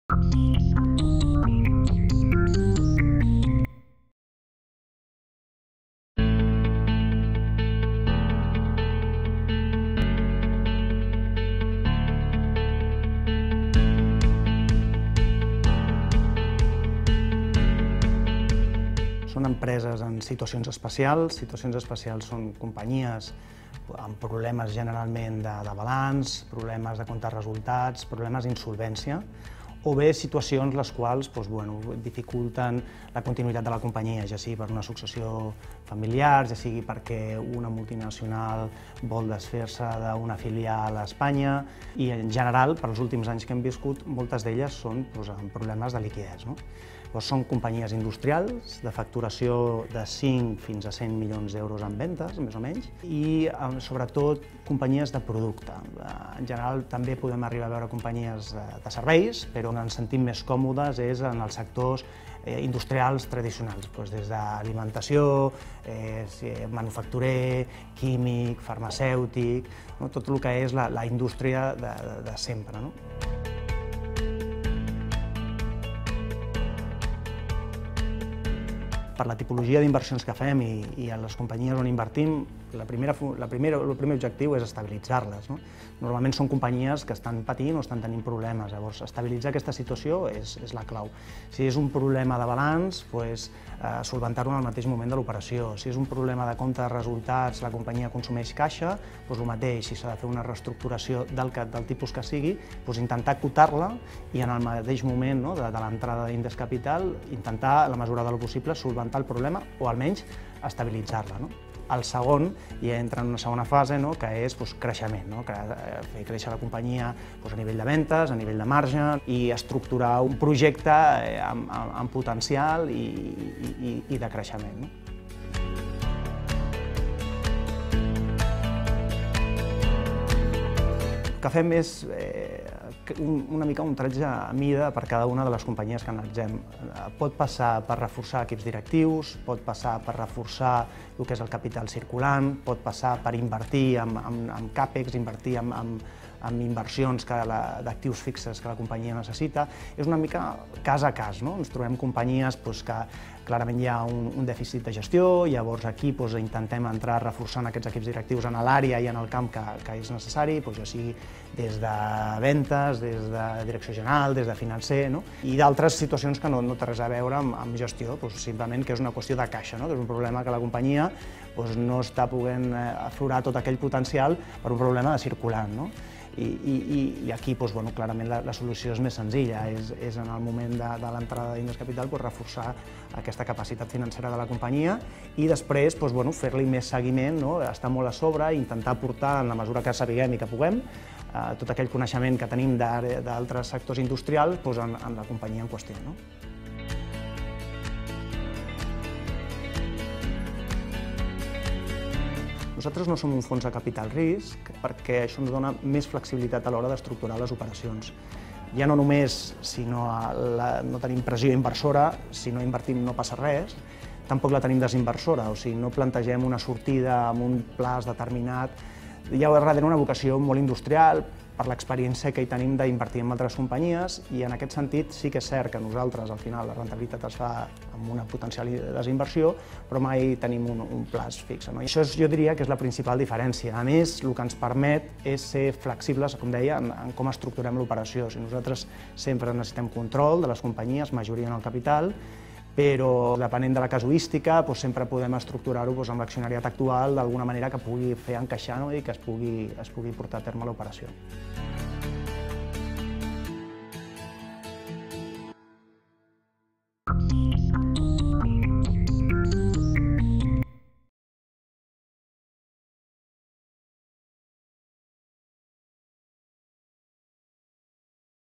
Els reptes del capital risc i el creixement
Joan Trenchs, investment executive d'Indes Capital parla de les fases de la inversió i explica per què no es defineixen com un fons de capital risc sinó com un grup inversor amb vocació industrial.